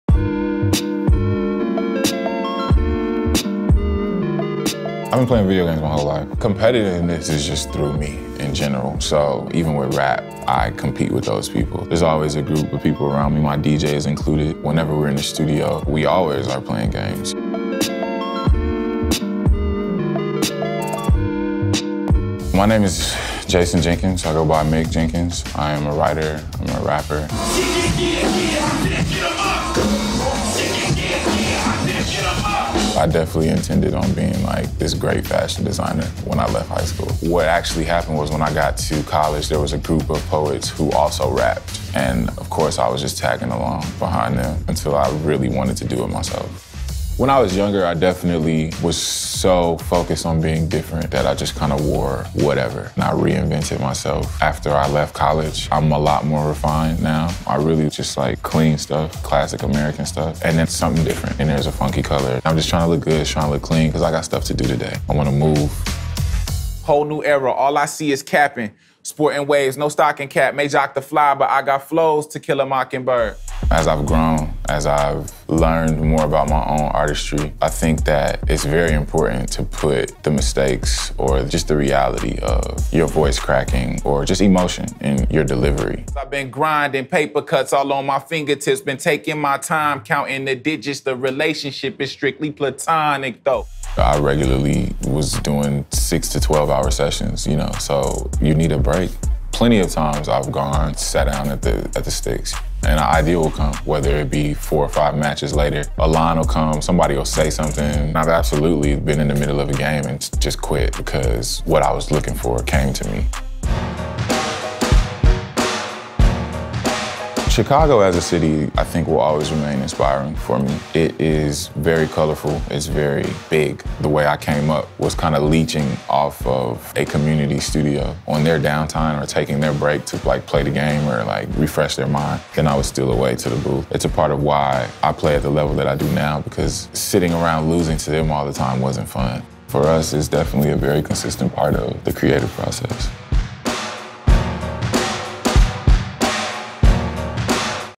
Player Two with Mick Jenkins
The Chicago MC laces up his Puma RS-X sneakers and shows us how video games play an integral part in his recording process.